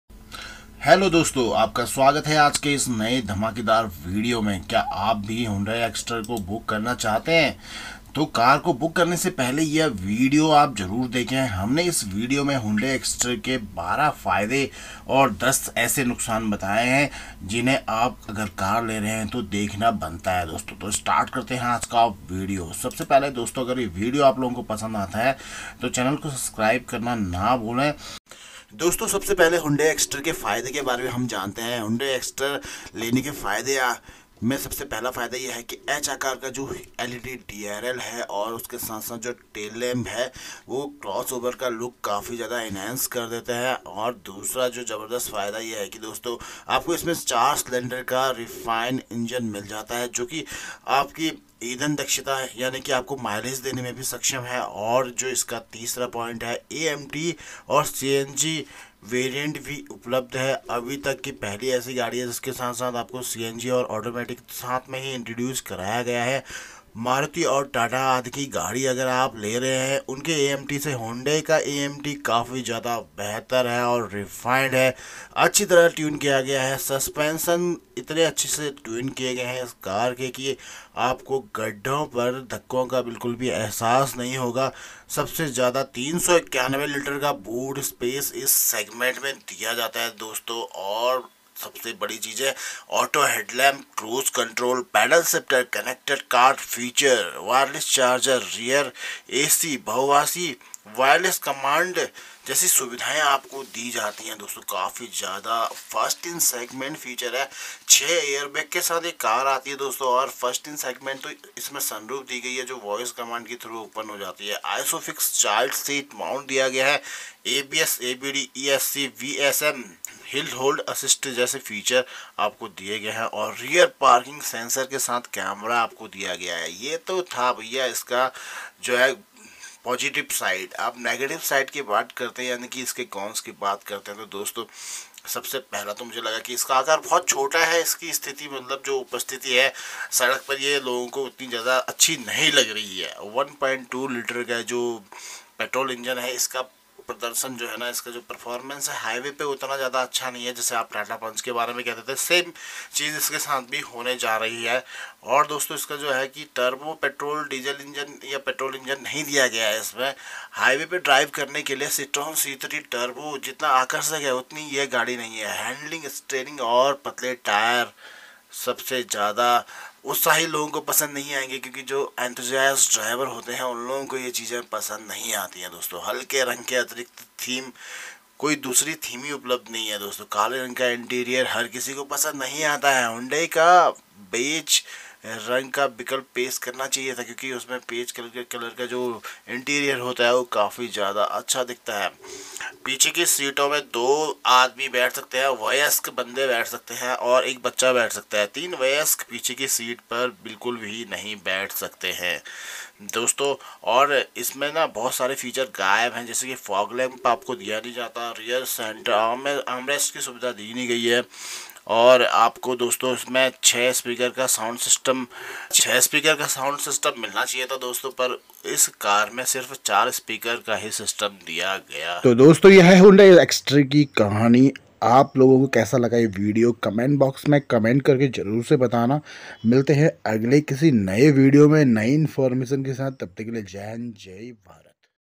Exploring Hyundai Exter: Pro’s and Con’s in Detail @meramotor1
Are you considering the Hyundai Exter? In this video, we dive deep into the Pro’s and Con’s of this remarkable vehicle. Discover whether it's the right choice for you!